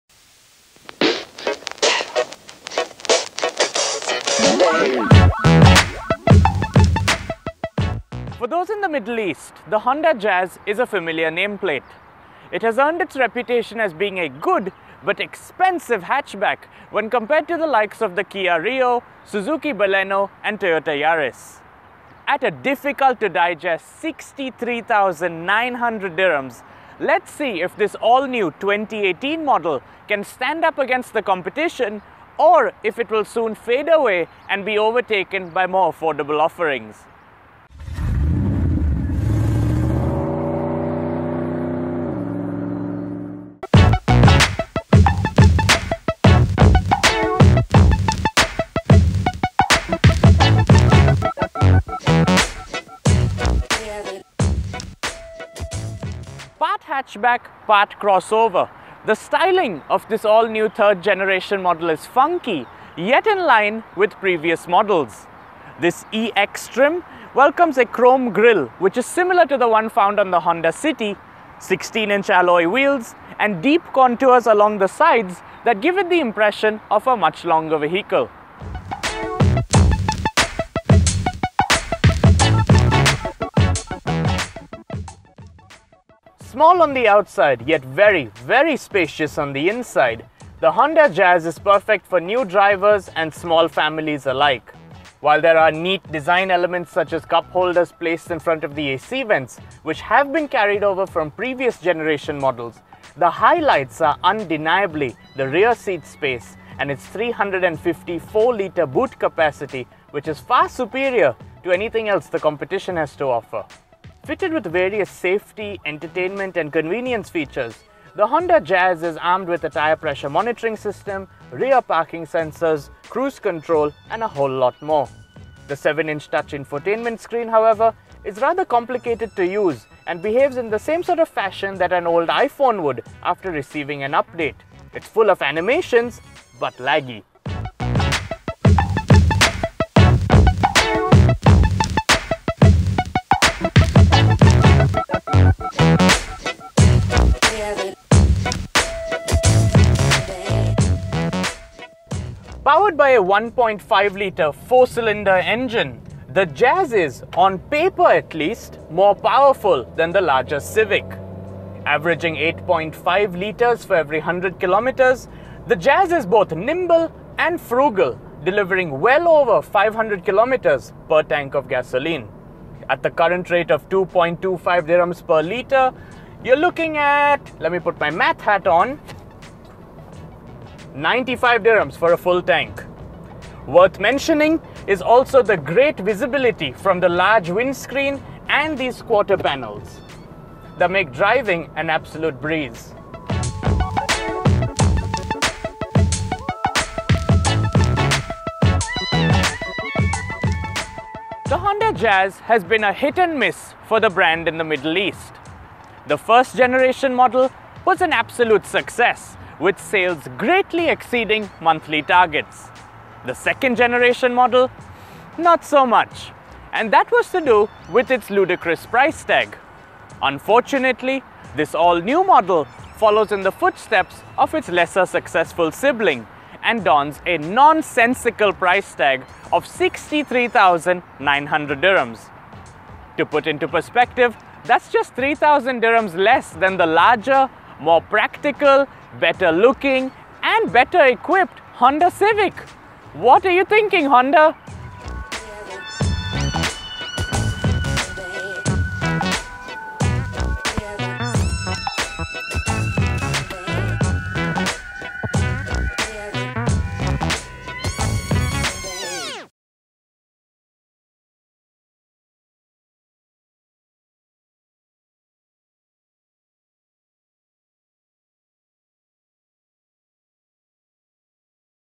Honda Jazz 2018 Review | YallaMotor.com
The Honda Jazz is a Japanese hatchback that has built its reputation as being a reliable and good looking family car in the UAE. Although on the pricier side of the spectrum, it offers plenty of room on the inside and is more powerful than the larger Civic on paper.

To get a better understanding of how the Jazz compares with its rivals, read our comprehensive comparative report

For those on the look out for a used Honda, check out